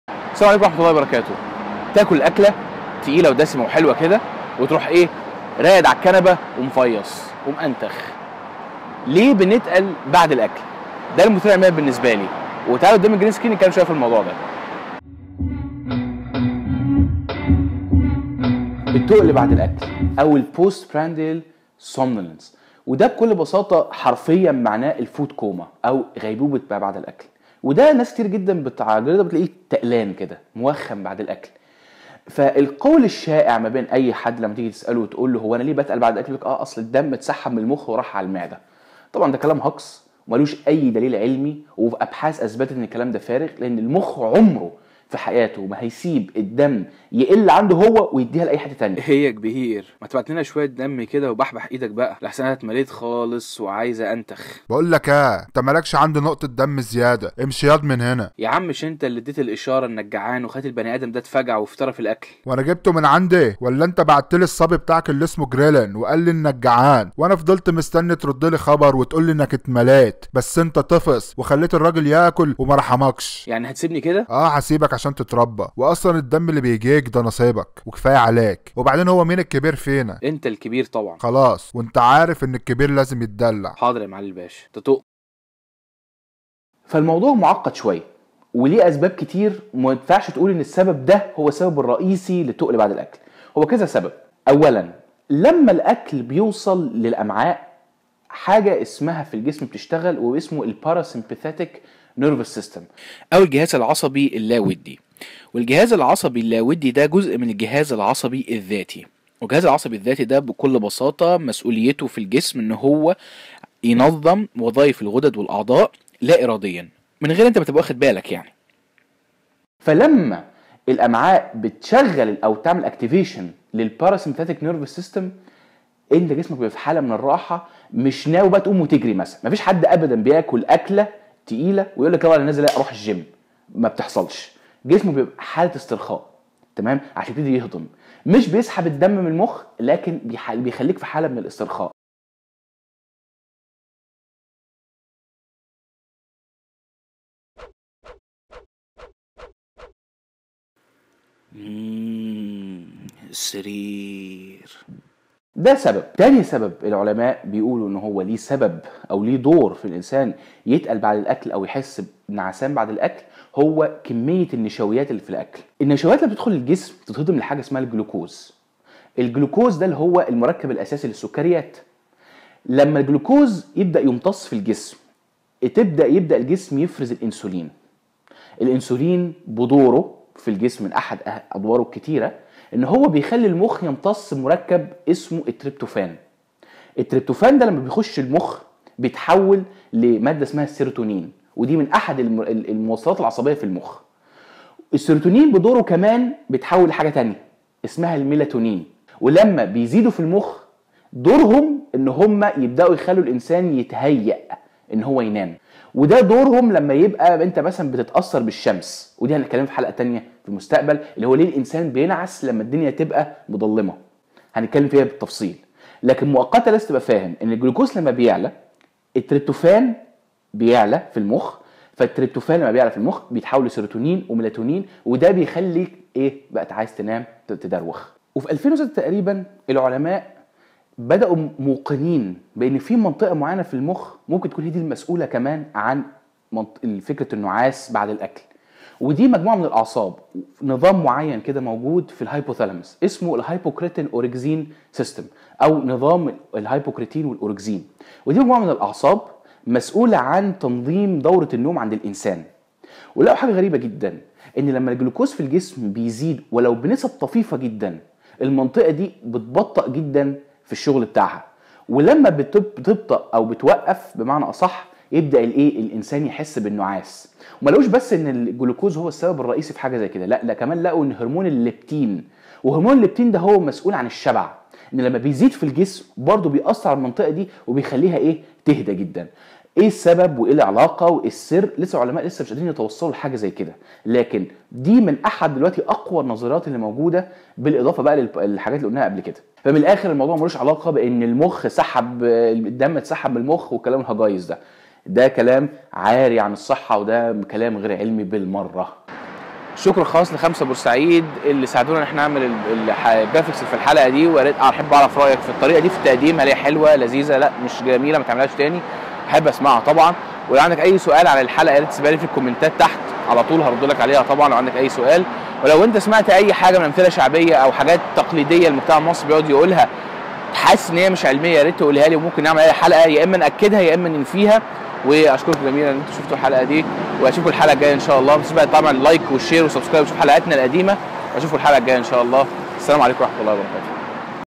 لماذا ننعس بعد الأكل؟ Postprandial somnolence
في الحلقة ده هانشرح ليه الإنسان بيتقل بعد الأكل .. و بنصحح أسطورة " إن الدم بيتسحب من المخ" ... شوف الحلقة عشان تعرف التفاصيل العلمية الشيقه ورا الظاهرة ده!

أقول مأثورة:
تعلموا العلم, و تعلموا السكينة و الوقار
- عمربن الخطاب (رضي الله عنه)
شكر خاص لخمسة بورسعيد مشاركتي في الحلقة ده!